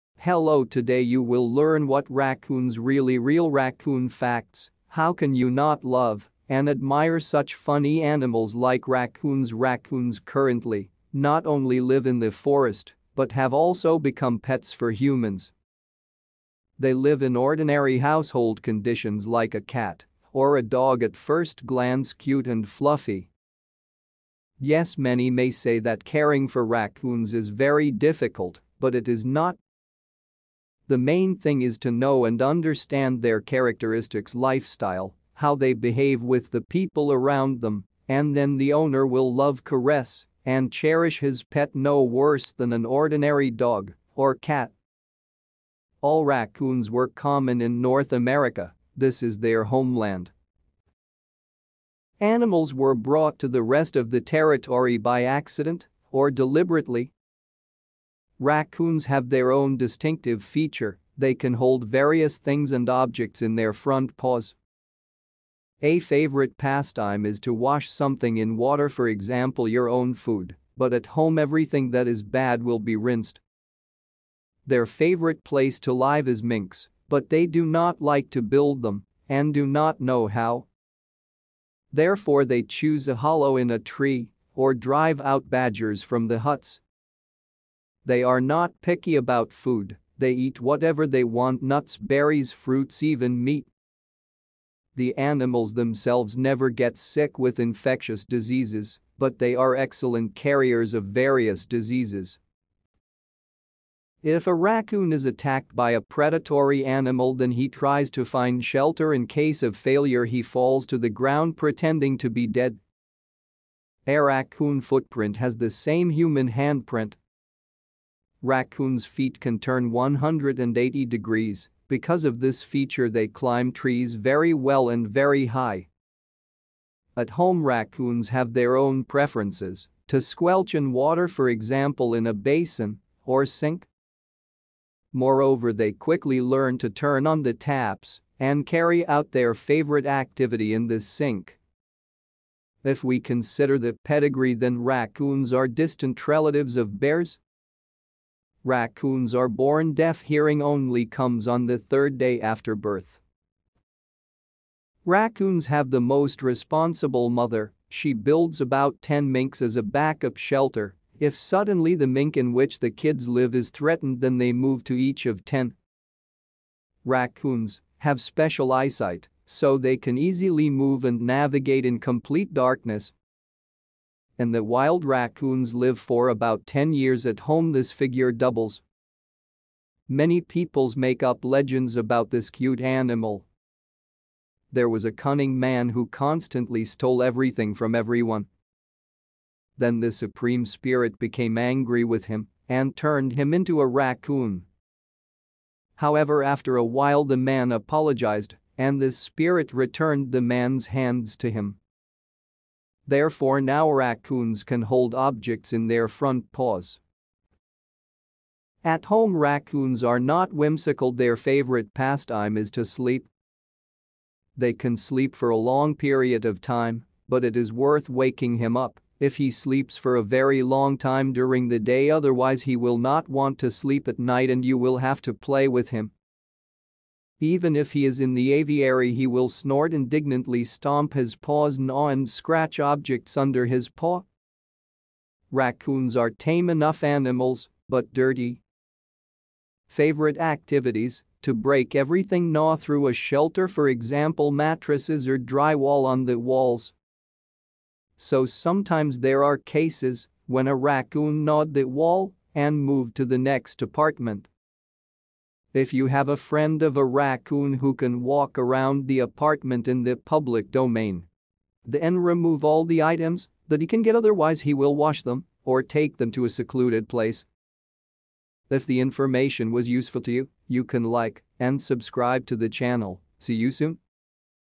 WHAT ARE RACCOONS REALLY REAL RACCOON FACTS!
REAL RACCOON FACTS! How can you not love and admire such funny animals like raccoons? Raccoons currently not only live in the forest, but have also become pets for humans. They live in ordinary household conditions, like a cat or a dog, at first glance, cute and fluffy. Yes, many may say that caring for raccoons is very difficult, but it is not. The main thing is to know and understand their characteristics, lifestyle, how they behave with the people around them, and then the owner will love, caress and cherish his pet, no worse than an ordinary dog or cat. All raccoons were common in North America - this is their homeland. Animals were brought to the rest of the territory by accident or deliberately. Raccoons have their own distinctive feature - they can hold various things and objects in their front paws. A favorite pastime is to wash something in water, for example, your own food, but at home everything that is bad will be rinsed. Their favorite place to live is minks, but they do not like to build them and do not know how. Therefore, they choose a hollow in a tree or drive out badgers from the huts. They are not picky about food - they eat whatever they want: nuts, berries, fruits, even meat. The animals themselves never get sick with infectious diseases, but they are excellent carriers of various diseases. If a raccoon is attacked by a predatory animal, then he tries to find shelter, in case of failure he falls to the ground, pretending to be dead. A raccoon footprint has the same human handprint. Raccoons' feet can turn one hundred and eighty degrees, because of this feature, they climb trees very well, and very high. At home, raccoons have their own preferences - to squelch in water, for example, in a basin or sink. Moreover, they quickly learn to turn on the taps and carry out their favorite activity in the sink. If we consider the pedigree, then raccoons are distant relatives of bears. Raccoons are born deaf, hearing only comes on the third day after birth. Raccoons have the most responsible mother - she builds about 10 minks as a backup shelter, if suddenly the mink in which the kids live is threatened, then they move to each of ten. Raccoons have special eyesight, so they can easily move and navigate in complete darkness. In the wild, raccoons live for about 10 years; at home, this figure doubles. Many peoples make up legends about this cute animal. There was a cunning man who constantly stole everything from everyone. Then the Supreme Spirit became angry with him and turned him into a raccoon. However, after a while the man apologized and the spirit returned the man's hands to him. Therefore, now raccoons can hold objects in their front paws. At home, raccoons are not whimsical, their favorite pastime is to sleep. They can sleep for a long period of time, but it is worth waking him up if he sleeps for a very long time during the day, otherwise he will not want to sleep at night and you will have to play with him. Even if he is in the aviary, he will snort indignantly, stomp his paws, gnaw and scratch objects under his paw. Raccoons are tame enough animals, but dirty. Favorite activities - to break everything, gnaw through a shelter, for example, mattresses or drywall on the walls. So sometimes there are cases when a raccoon gnawed the wall and moved to the next apartment. If you have a friend of a raccoon who can walk around the apartment in the public domain, then remove all the items that he can get, otherwise he will wash them or take them to a secluded place. See you soon.